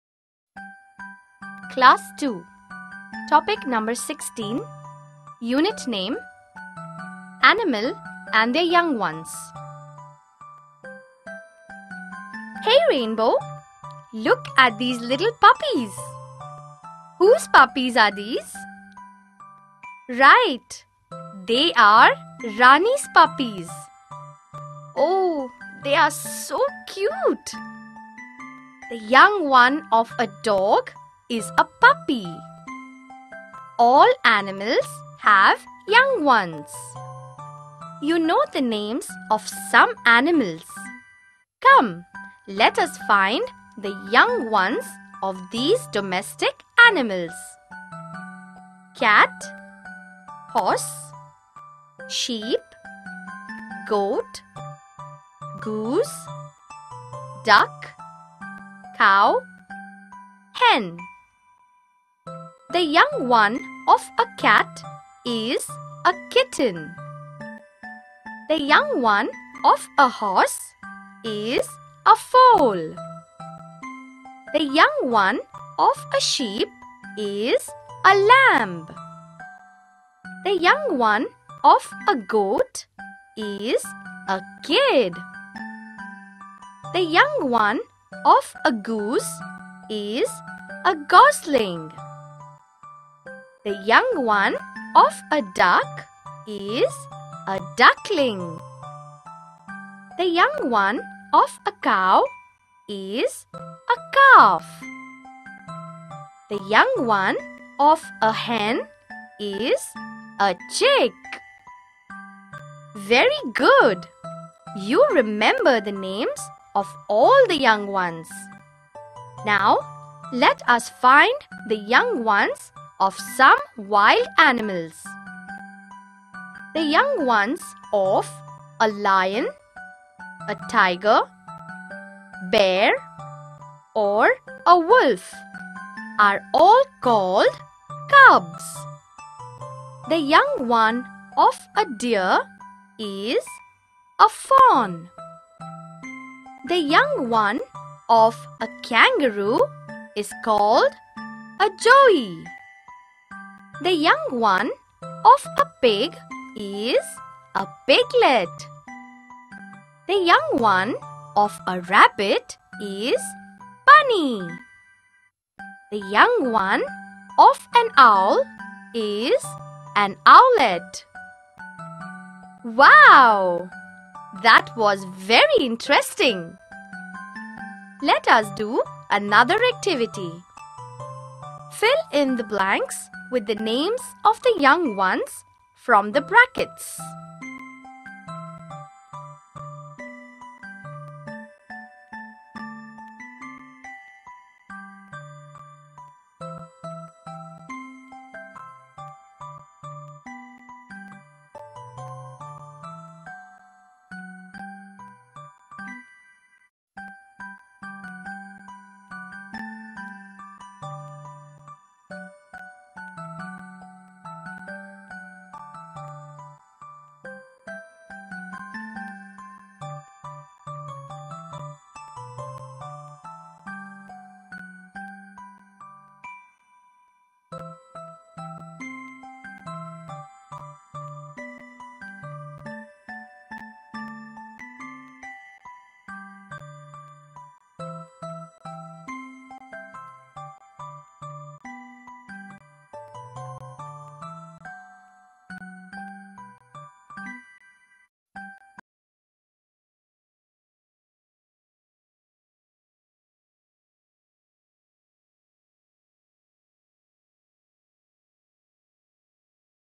Learn Grade 2 - English Grammer - Animal and their young ones
In this video you get to Learn English Grammer - Animal and their young ones Grade 2.